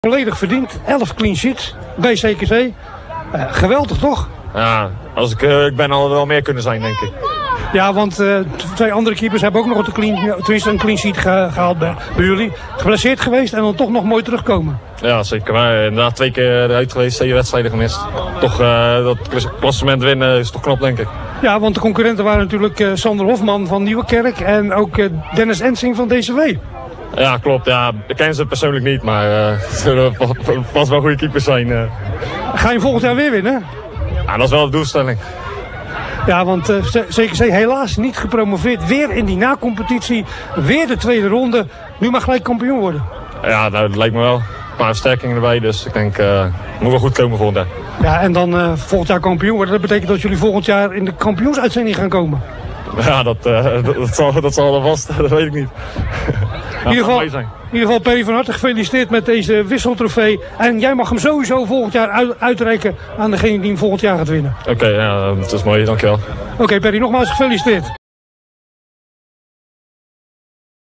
CKC-keeper Perry Heijser neemt eerste Clean Sheettrofee in ontvangst
CKC-doelman Perry Heijser hield afgelopen jaar maar liefst 11 maal zijn doel schoon. Genoeg in ieder geval om de IJssel-sport Clean-sheettrofee te veroveren. Verslaggever Kasper Luchtmeijer toog richting Sportpark 't Veer om Heijser de bokaal te overhandigen en een paar keepershandschoenen van 100%Voetbal Capelle.